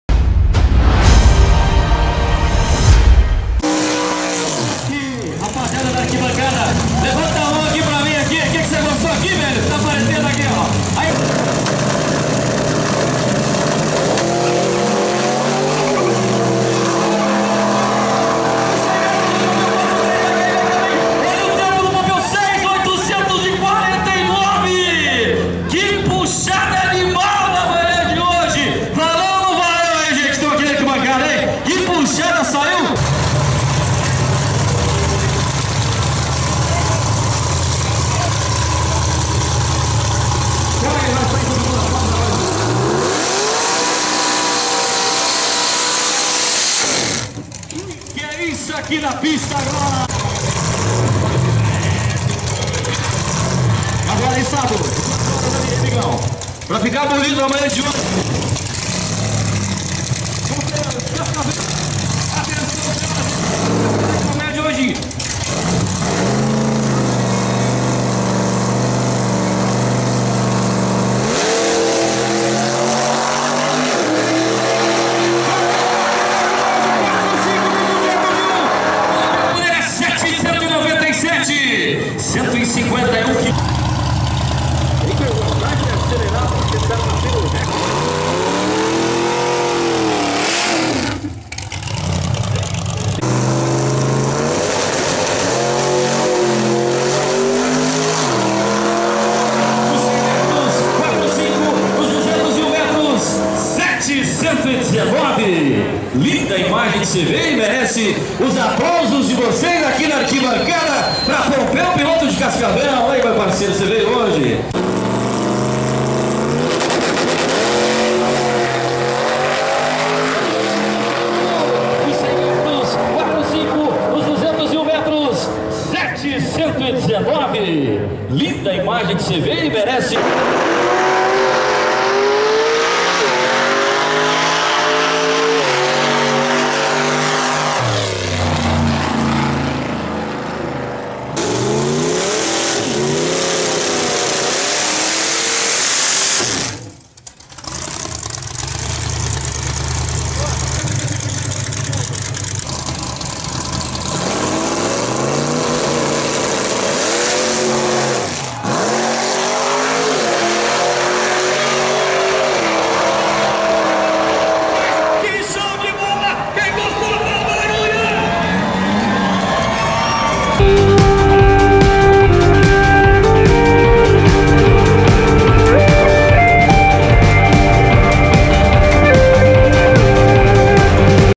Desafio Slick - Kombi V8 - RENATO CESAR POMPEU - 6,8s
Paranaense e Sul Brasileiro de Arrancada 2019
Toledo PR